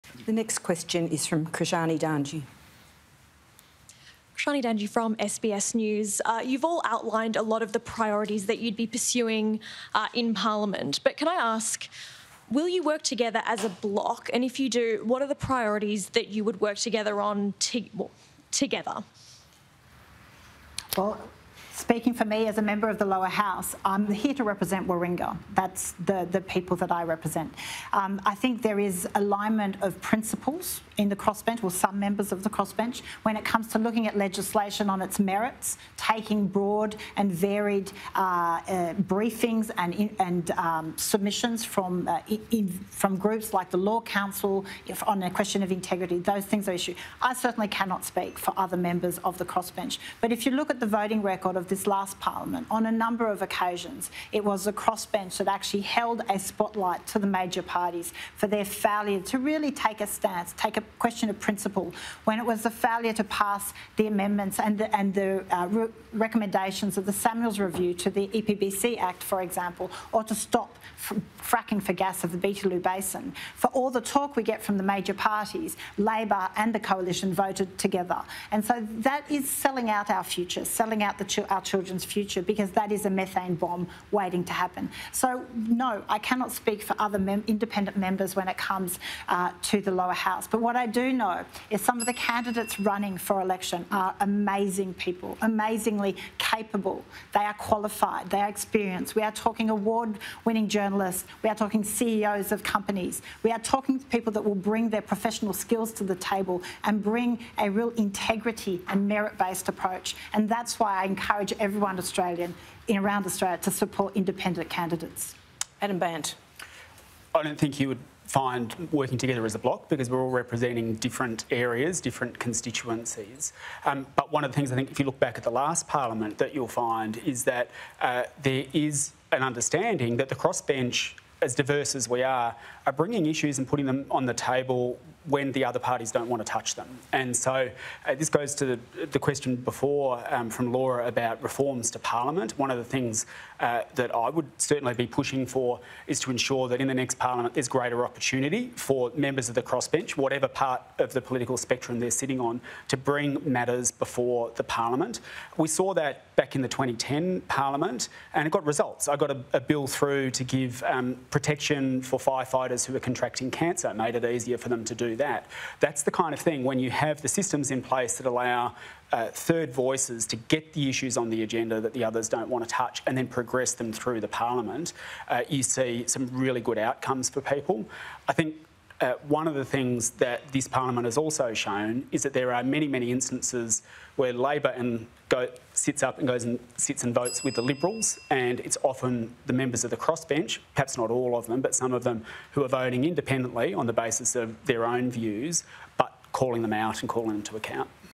Some independent candidates running for election are 'amazingly capable': Zali Steggall
Independent MP Zali Steggall says some independent candidates running in this election are “amazingly capable” people who are qualified and experienced.

It comes as Ms Steggall was asked at a National Press Club event if independents would work as a "bloc" in the parliament.

“I cannot speak for other independent members when it comes to the Lower House, but what I do know is some of the candidates running for election are amazing people, amazingly capable," she said.

“They are qualified, they are experienced; we are talking award-winning journalists, we are talking CEOs of companies, we are talking people who will bring their professional skills to the table and bring a real integrity and merit-based approach."